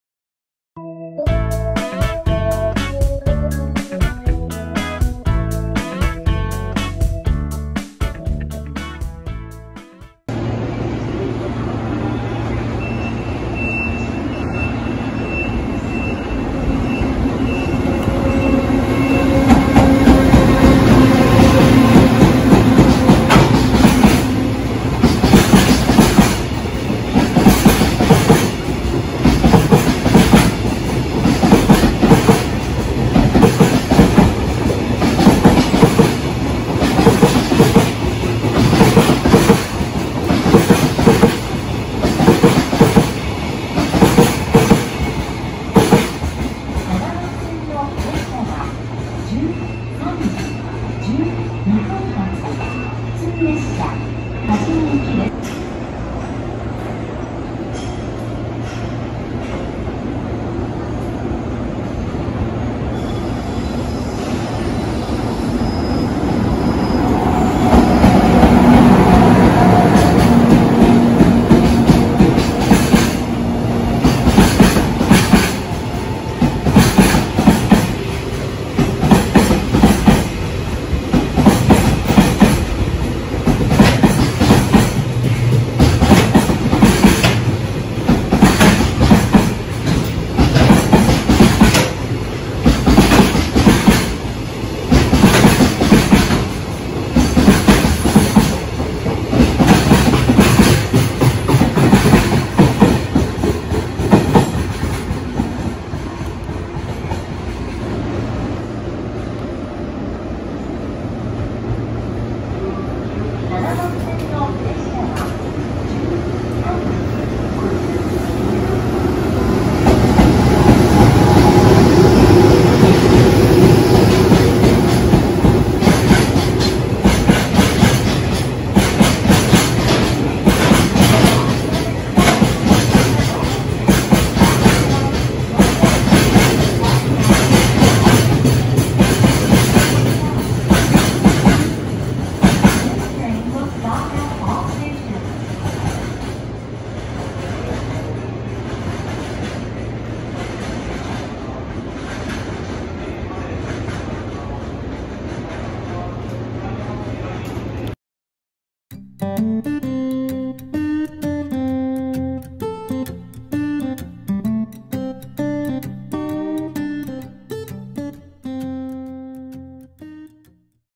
約4時間遅れの1653レを含む、貨物列車3本編集。
⚪️撮影場所=JR名古屋駅8番線

①【1653レ・EF210-103・コンテナ輸送列車】
▲1353レ=名古屋（タ）6:52発→稲沢7:10着
◎1653レ=稲沢9:10発→多治見10:09着
※表示は定刻の時刻を参照してます。

②【81レ・EF64-1028・コンテナ輸送列車】
▲2095レ=名古屋（タ）12:11発→稲沢12:34着
◎81レ=稲沢13:04発→南松本18:24着
▲83レ=南松本22:05発→北長野7:10着

③【5875レ・EH200-10・石油輸送列車】
▲5282レ=塩浜11:12発→稲沢13:02着
◎5875レ=稲沢13:36発→南松本18:40着